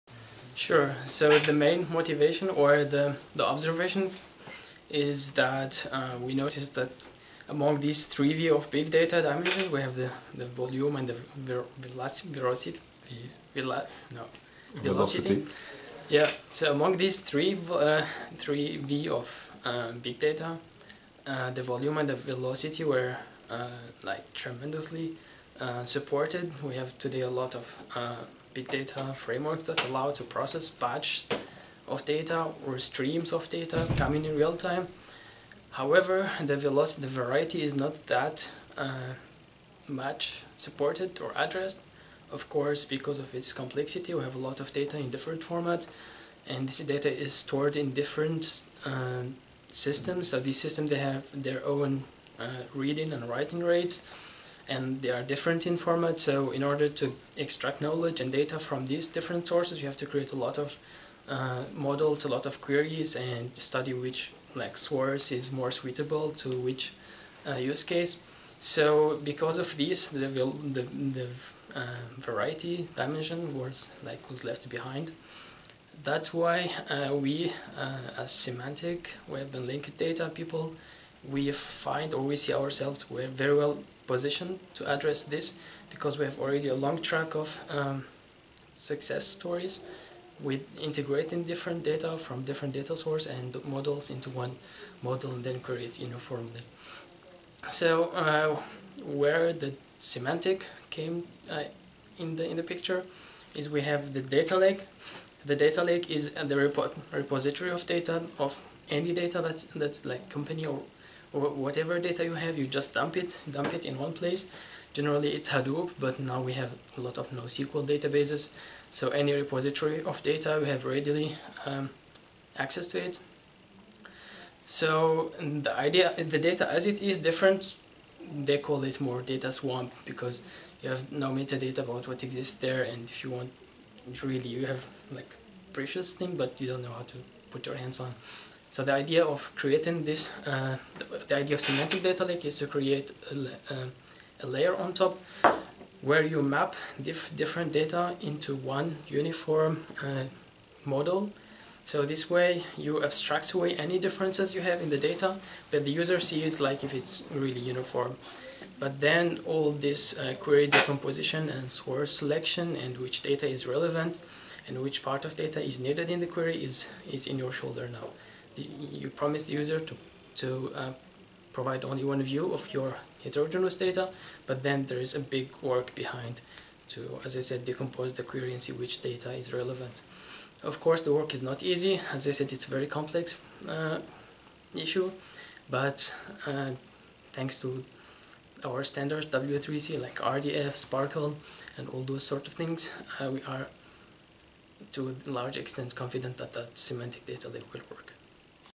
Can You Explain the Concept of the Semantic Data Lake?
Two of the three Vs of big data - volume and velocity - have had a lot of attention and seem largely solved. Not so the problem of variety, that is, handling differenet and diverse sources and types of data. Mohamed Nadjib Mami explains how the Big Data Europe Integrator Platform solves the problem by abstracting all data into a semantic layer - the Semantic Data Lake.